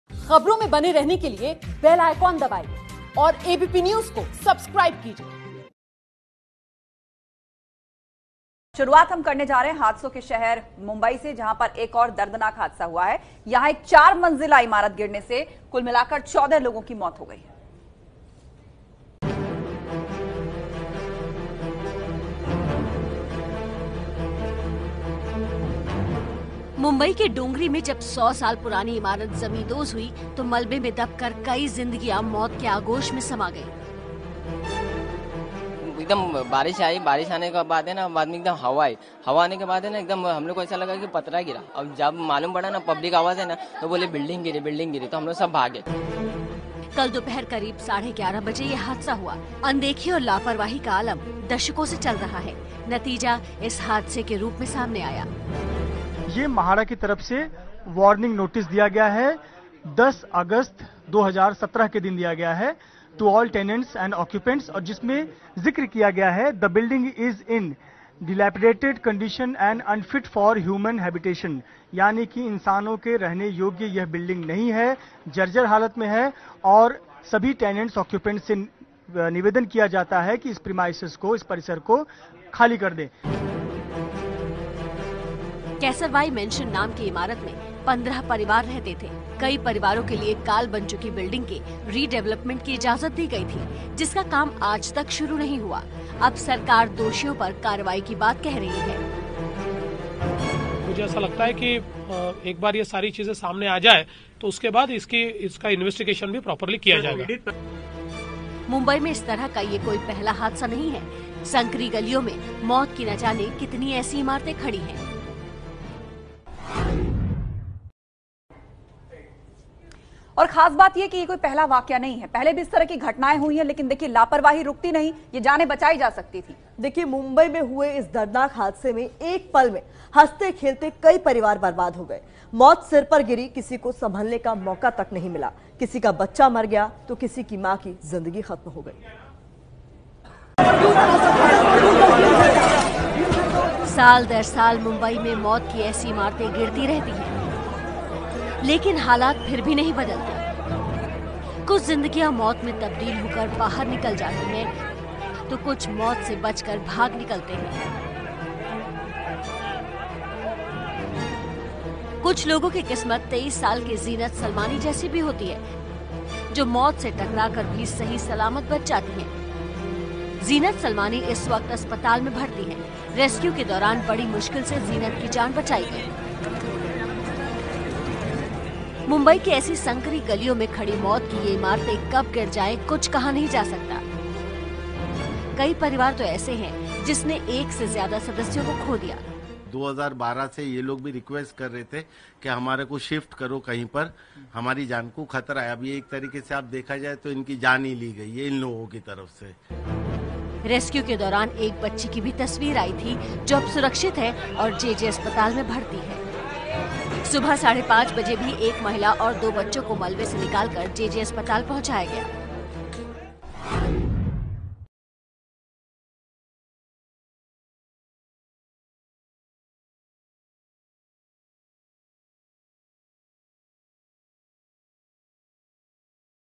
मुंबई के डोंगरी में सौ साल पुरानी इमारत गिरी, 14 लोगों की मौत | ABP News Hindi
मुंबई के डोंगरी में सौ साल पुरानी इमारत गिरी, 14 लोगों की मौत |